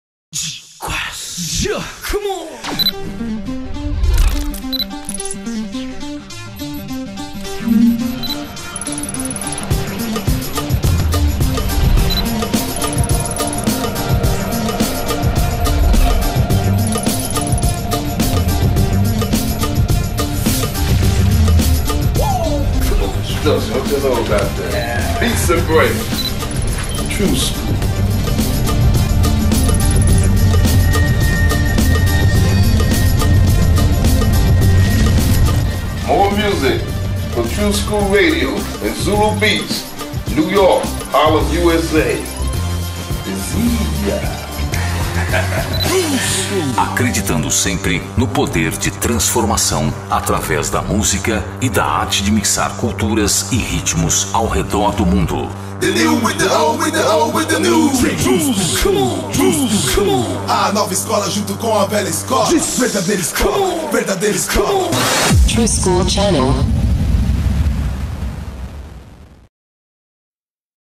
Canal True School Brasil & Afrika Bambaataa
Canal de transmissão ao vivo pela internet.
DJs mixando ao vivo direto de seus estudios em diversas cidades , estados e países.
Conexão New York True School Musical Show
]Zulu Beats - Harlem - USA